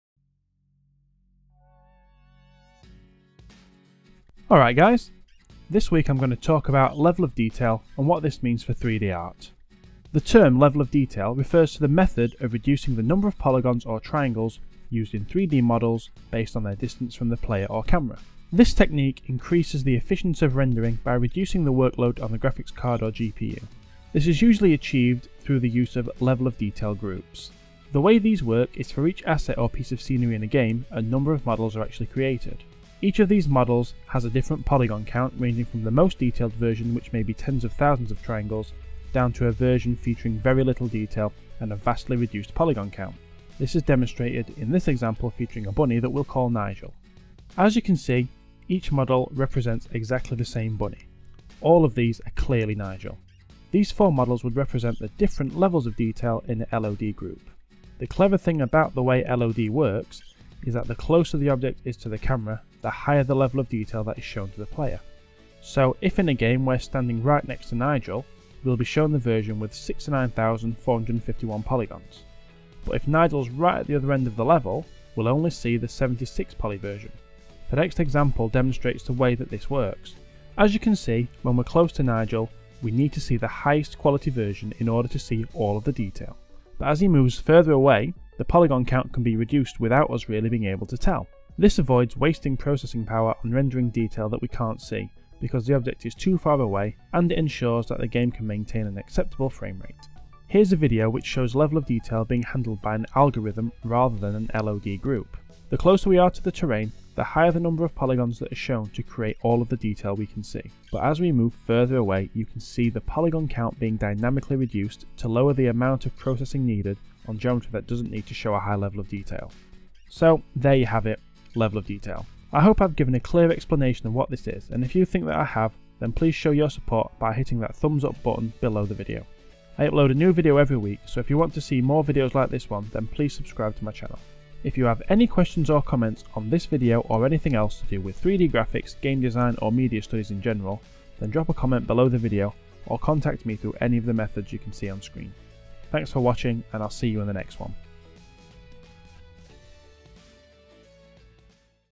What is LOD? (Level of Detail & LOD Groups)
What is Level of Detail? What is LOD? In this video I explain what level of detail is and how it is used in computer games. I also give some examples of LOD groups.